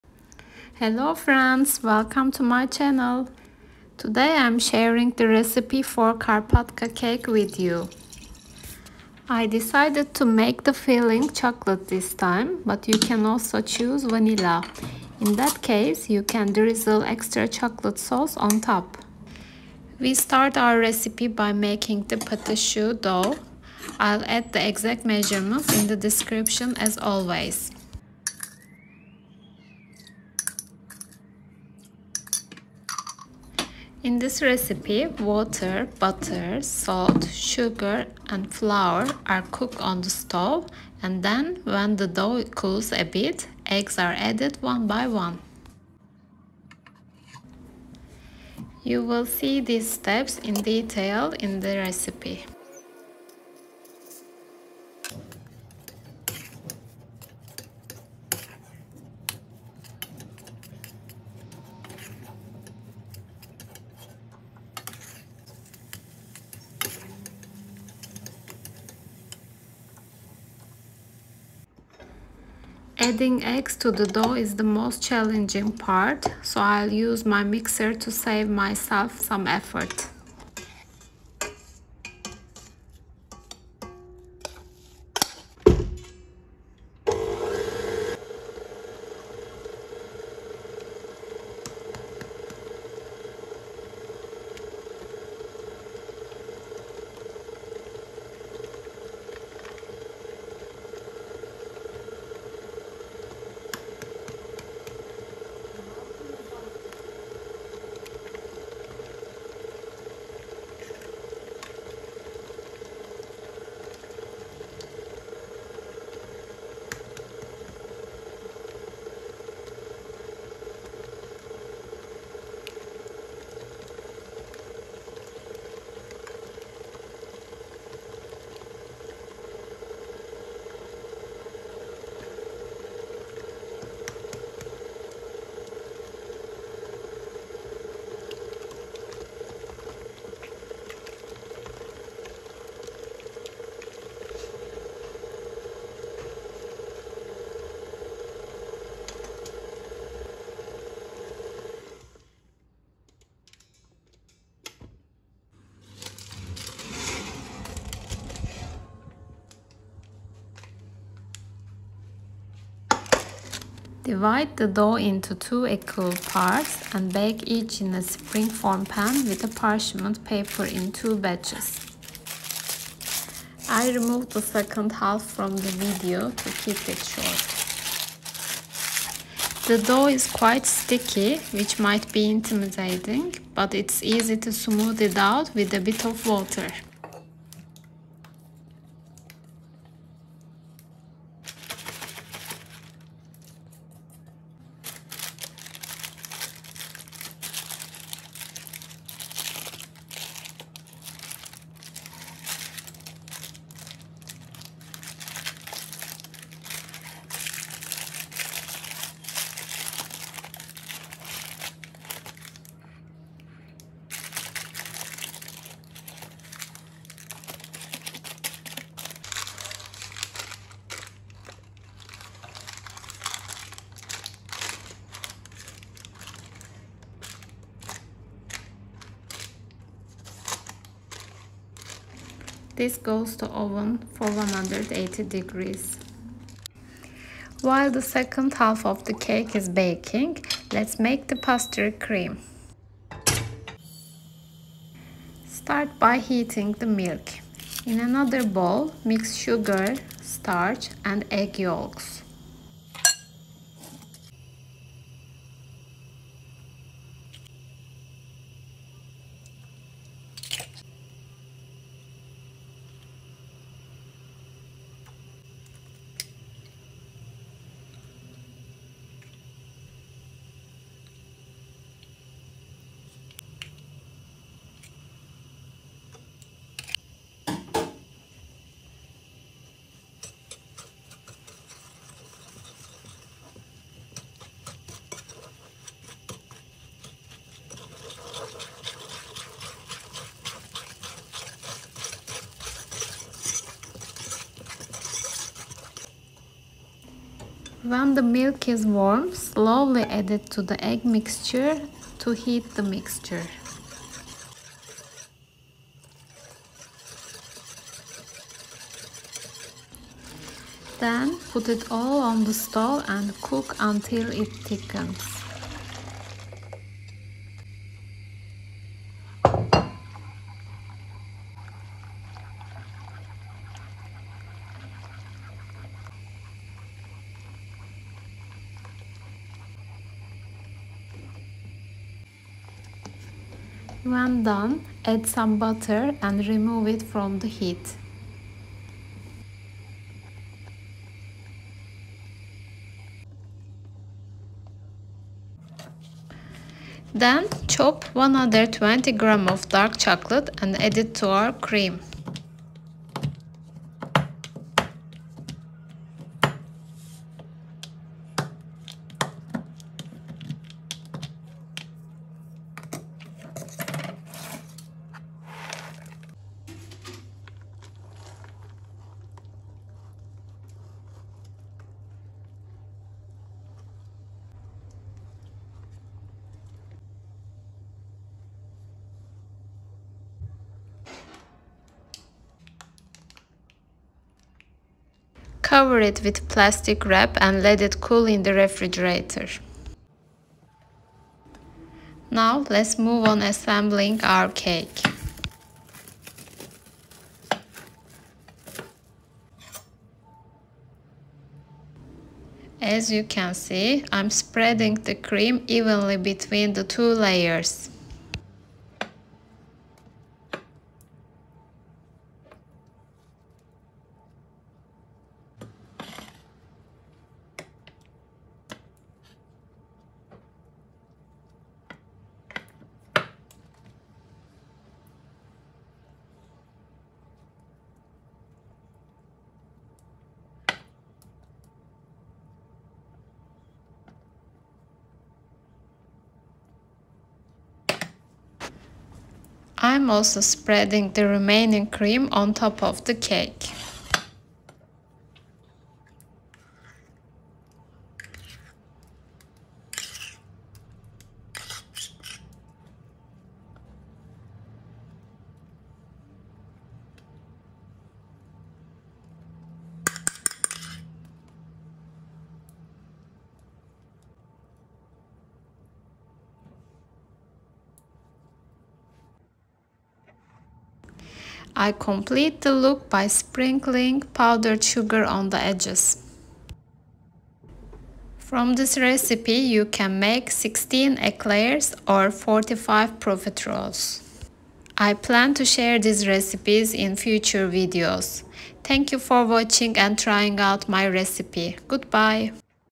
Giant profiterole aka Karpatka cake😋🎂🍫
Ingredient list:
Pate a  choux:
240g flour
25g powdered sugar
4g salt
100g butter
175g flour
Four large eggs

Vanilla Pastry Cream
480g milk
One packet of vanilla
Five large egg yolks
133g sugar
40g wheat starch
50g butter
120g bitter chocolate

Today, I'm sharing the recipe for Karpatka cake with you. I decided to make the filling chocolate this time, but you can also choose vanilla. In that case, you can drizzle extra chocolate sauce on top. We start our recipe by making the pate choux dough. I'll add the exact measurements in the description as always. In this recipe, water, butter, salt, sugar, and flour are cooked on the stove, and then, when the dough cools a bit, eggs are added one by one. You'll see these steps in detail in the recipe. Adding eggs to the dough is the most challenging part, so I'll use my mixer to save myself some effort. The dough is quite sticky, which might be intimidating, but it's easy to smooth it out with a bit of water. Divide the dough into two equal parts and bake each in a springform pan with parchment paper in two batches. I removed the second half from the video to keep it concise. While the second half of the cake is baking, let's make the pastry cream. Start by heating the milk. In another bowl, mix sugar, starch, and egg yolks. When the milk is warm, slowly add it to the egg mixture and heat the mixture. Then, put it all on the stove and cook until it thickens. When done, add some butter and remove it from the heat. Then, chop 120g of dark chocolate and add it to our cream. Cover it with plastic wrap and let it cool in the refrigerator. Now, let's move on to assembling our cake. As you can see, I'm spreading the cream evenly between the two layers. I'm also spreading the remaining cream on top of the cake. I complete the look by sprinkling powdered sugar on the edges. From this recipe, you can make 16 eclairs or 45 profiteroles. I plan to share these recipes in future videos. Goodbye!